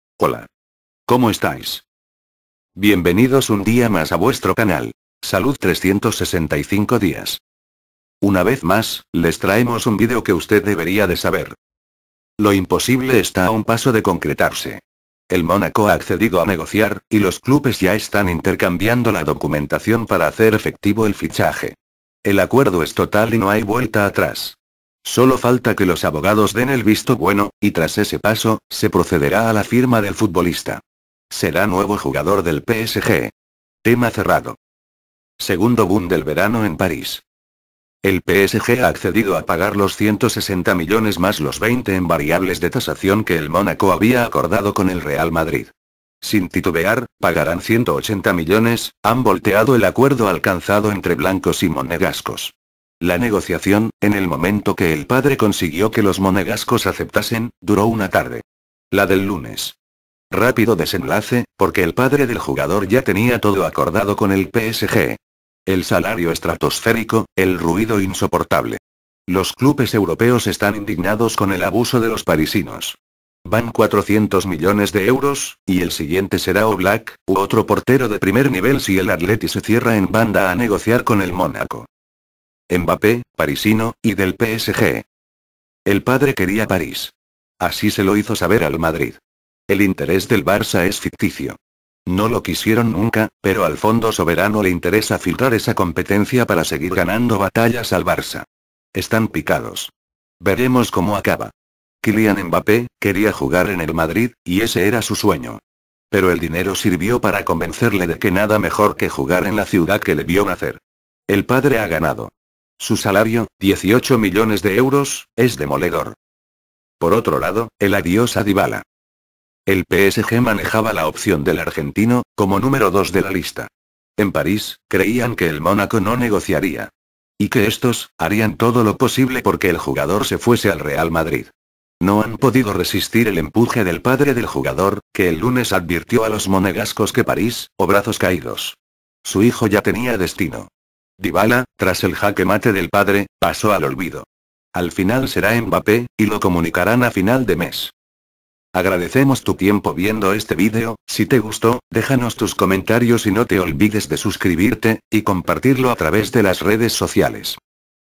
😱😱FICHADO!!! Como fue y SUELDO de MBAPPE
¡¡Hola. ¿Cómo estais?!!.
Bienvenidos un dia mas a vuestro canal; salud 365 dias.
Una vez más, les traemos un video que usted deberia de saber.

Lo imposible está a un paso de concretarse. El Mónaco ha accedido a negociar, y los clubes ya están intercambiando la documentación para hacer efectivo el fichaje. El acuerdo es total y no hay vuelta atrás. sólo falta que los abogados den el visto bueno, y tras ese paso, se procederá a la firma del futbolista. Será nuevo jugador del PSG. Tema cerrado.

Segundo boom del verano en París.

El PSG ha accedido a pagar los 160 millones más los 20 en variables de tasación que el Mónaco había acordado con el Real Madrid. Sin titubear, pagarán 180 millones,  han volteado el acuerdo alcanzado entre blancos y monegascos. La negociación, en el momento que el padre consiguió que los monegascos aceptasen, duró una tarde. La del lunes. Rápido desenlace, porque el padre del jugador ya tenía todo acordado con el PSG. El salario estratosférico, el ruido insoportable. Los clubes europeos están indignados con el abuso de los parisinos. Van 400 millones de euros, y el siguiente será Oblak, u otro portero de primer nivel si el Atleti se cierra en banda a negociar con el Mónaco.

Mbappé, Parisino, y del PSG.

El padre quería París. Así se lo hizo saber al Madrid. El interés del Barça es ficticio. No lo quisieron nunca, pero al fondo soberano le interesa filtrar esa competencia para seguir ganando batallas al Barça. Están picados. Veremos como acaba. Kylian, quería jugar en el Madrid, ese era su sueño, pero el dinero sirvió para convencerle de que nada mejor que jugar en la ciudad que le vio nacer. El padre ha ganado. Su salario, 18 millones de euros, es demoledor.

Por otro lado, el Adiós a Dybala.

El PSG manejaba la opción del argentino, como número dos de la lista. En París, creían que el Mónaco no negociaría. Y que estos, harían todo lo posible porque el jugador se fuese al Real Madrid. No han podido resistir el empuje del padre del jugador, que el lunes advirtió a los monegascos que París, o brazos caídos. Su hijo ya tenía destino. Dybala, tras el jaque mate del padre, pasó al olvido. Al final será, Mbappé. Lo comunicarán a final de mes.